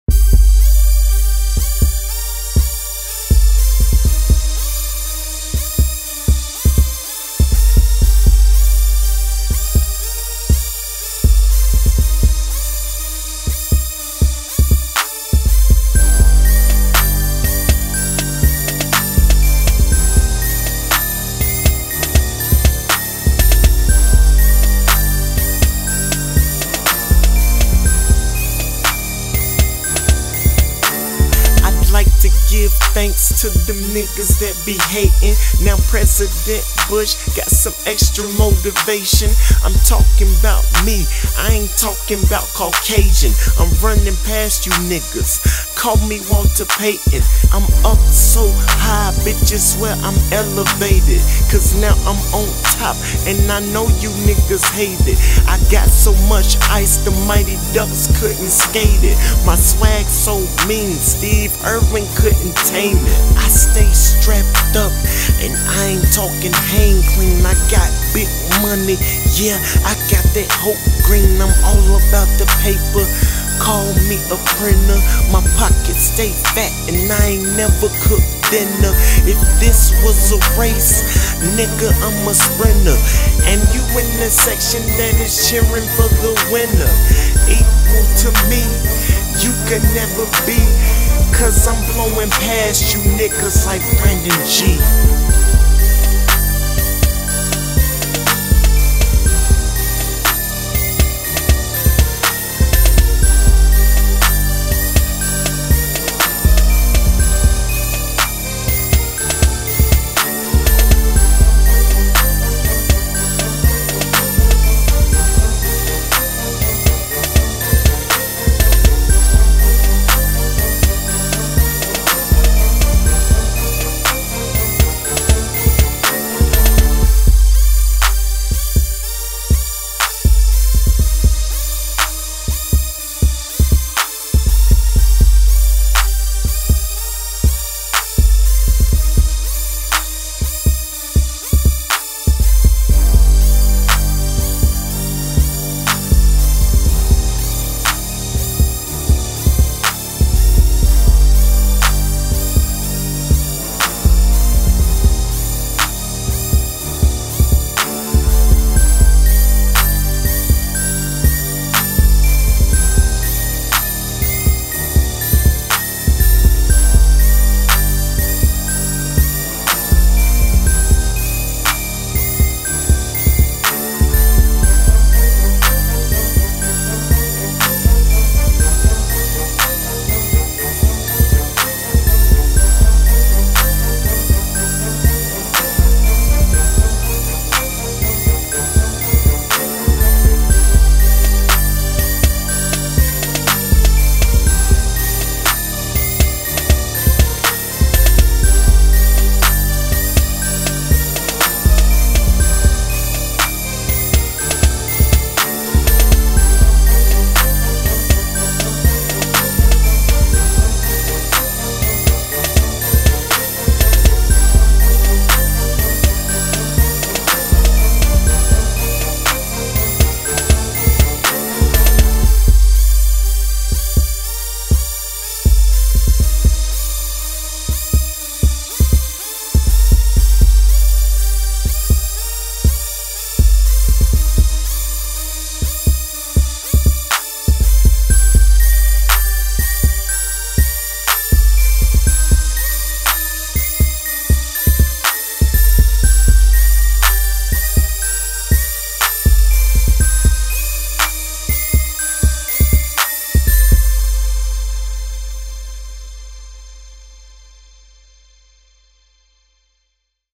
Motivation!
Josh Bush #25 Wake Forest Demon Deacons

...also produces his own beats and raps on them so here it is